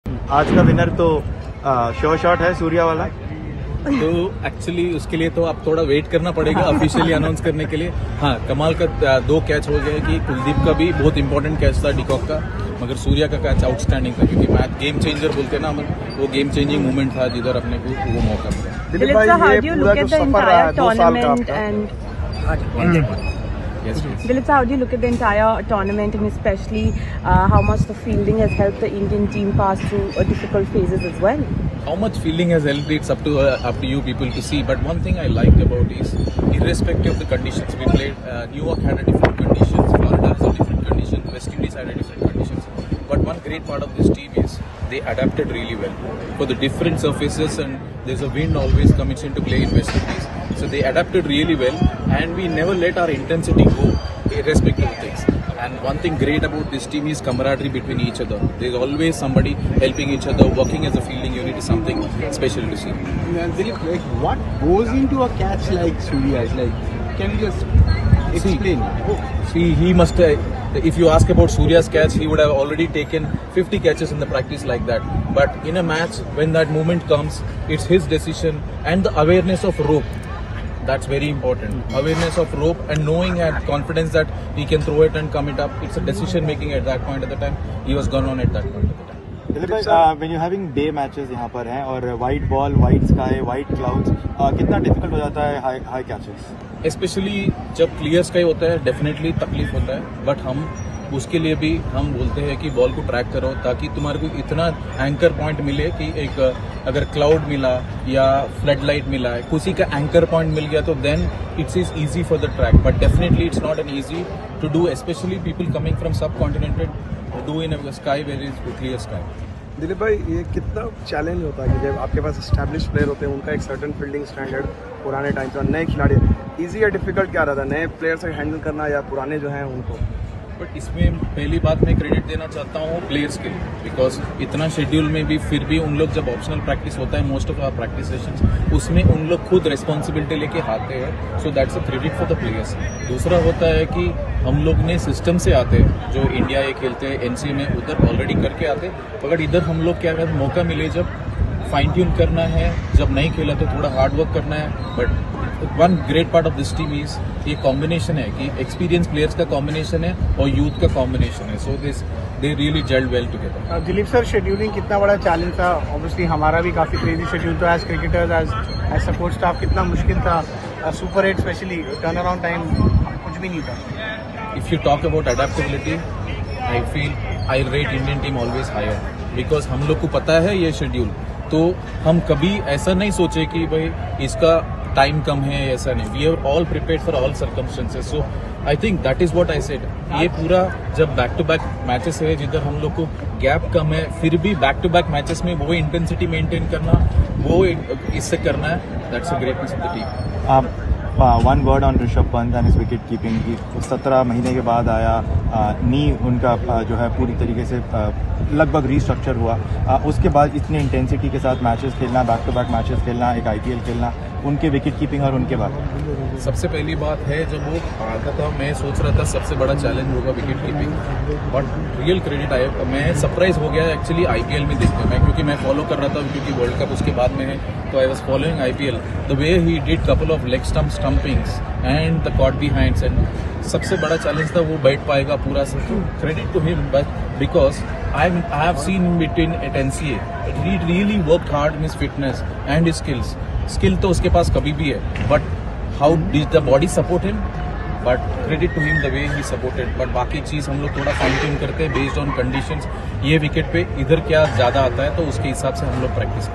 सूर्यकुमार यादव की बेहतरीन कैच के लिए क्या बोले फील्डिंग कोच टी दिलीप. Suryakumar yadav catch.
Surya Kumar Yadav Catch
suryakumar yadav catch